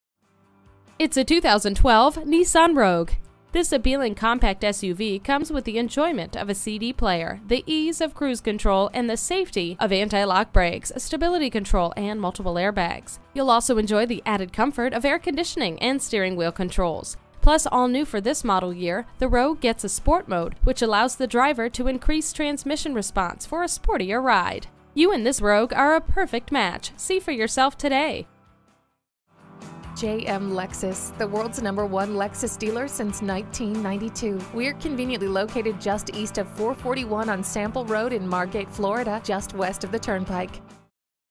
Used 2012 Nissan Rogue Margate FL Fort Lauderdale, FL P17073 - SOLD
Year: 2012
Make: Nissan
Model: Rogue
Trim: S
Engine: 2.5L 4 cyls
Transmission: CVT (Continuously Variable)
Color: Black
Interior: Gray
Mileage: 43081
Stock #: P17073
VIN: JN8AS5MT1CW611869
You are currently viewing a 2012 Nissan Rogue at the JM Lexus PreOwned Superstore. Thank you for your interest. We look forward to working for you. Stock #: P17073 Exterior: Black Interior: Gray Trim: S JM Lexus 5401 West Sample Rd Coconut Creek FL 33073

Address:
5350 W Sample Rd